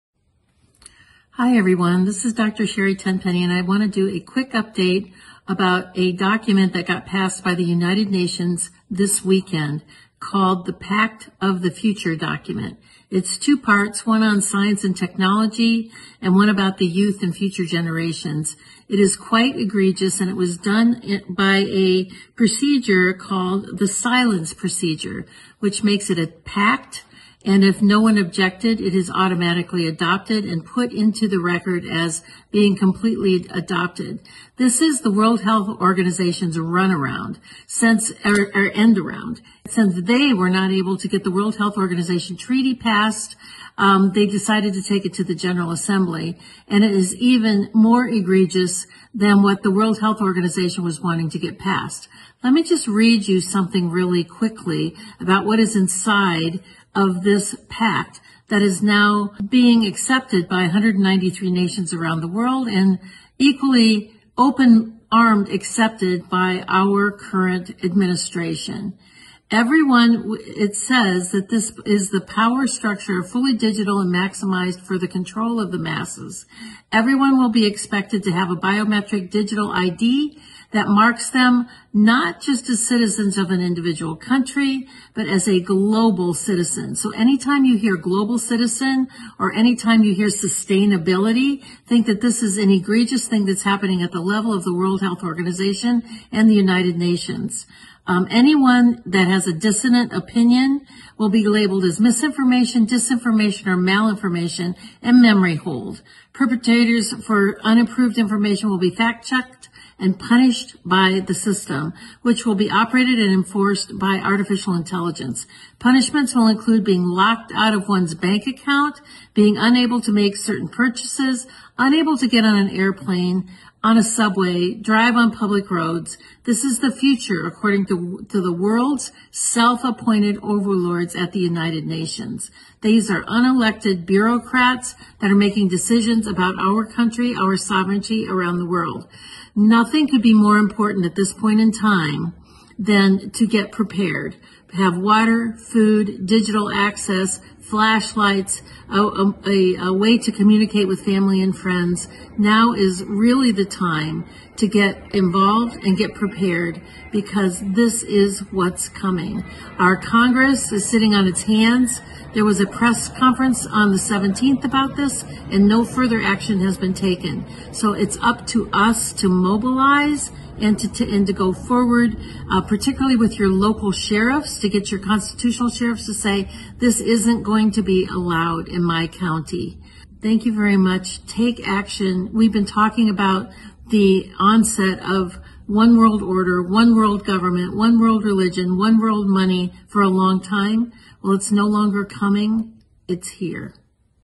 La Dr.ssa Sherry Tenpenny condivide i suoi pensieri sullo scellerato "PATTO PER IL FUTURO" dell'ONU
La Dott.ssa Tenpenny condivide i suoi pensieri sul tentativo dell'ONU di prendere il controllo degli Stati Uniti e del mondo e di sottoporci tutti sotto un controllo totalitario, parla del documento Patto del futuro delle Nazioni Unite. Riprende l'essenza del trattato fallito dell'Organizzazione Mondiale della Sanità. Pone le basi per l'ID digitale biometrico. Include anche la censura di massa delle informazioni non approvate.